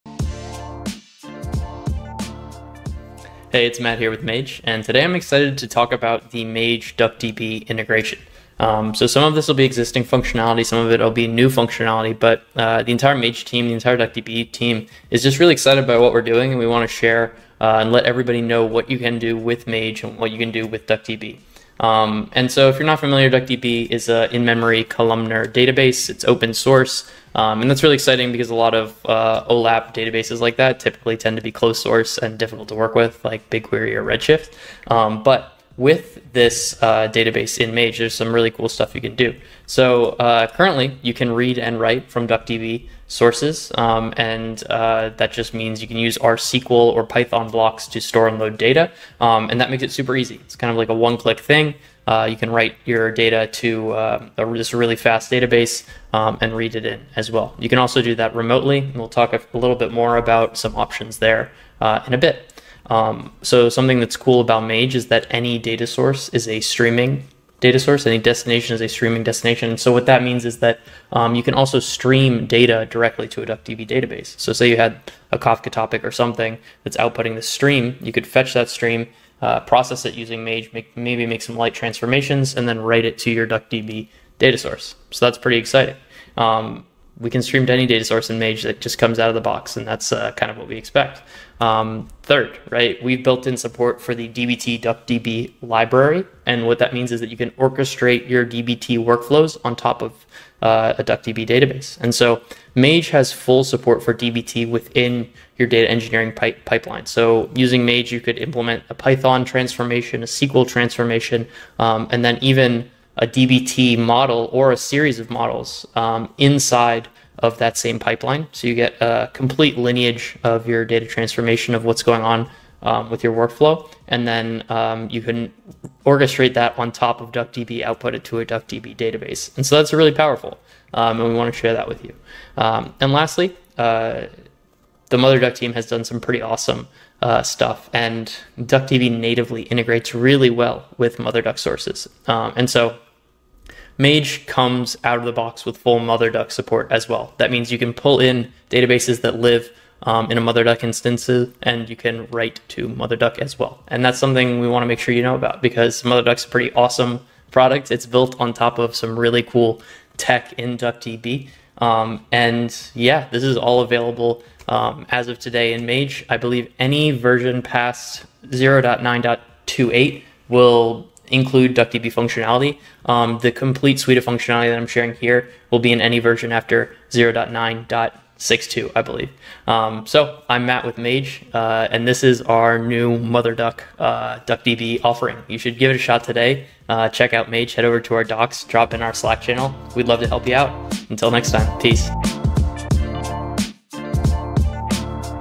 Magic Update: Mage & MotherDuck 🧙‍♂️🦆
🦆 Coming to a data pipeline near you... Mage & MotherDuck!

🤓 Mage has integrated with DuckDB for a few months now, but today we're rolling out full MotherDuck support! That means the ability to read and write from MotherDuck databases using Mage's built-in Data Loaders & Exporters!

🏎️ As always, if you can write it in Python or SQL, Mage can handle it! This update just provides easy-to-use templates for you to get started as fast as possible!

⚡ Be sure to give it a try today!